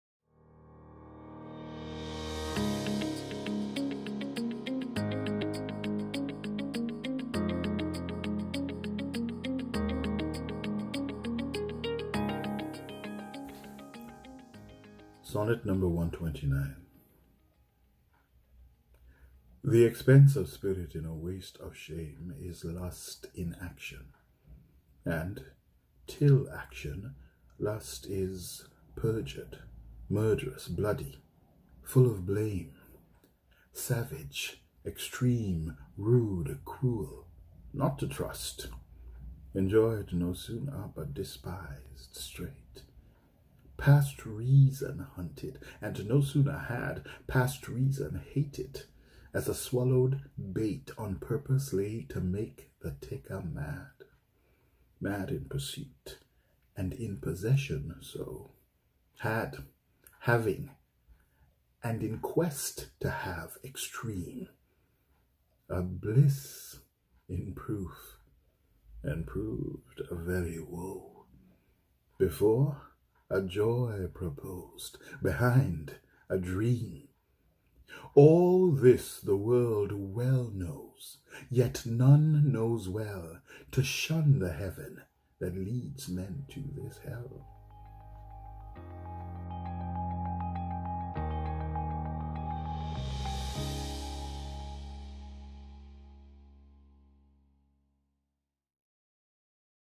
MCT | SONNETS: Sonnet 129, Anthon Mondesir
Sonnet 129, by William Shakespeare. Performned by Anthon Mondesir.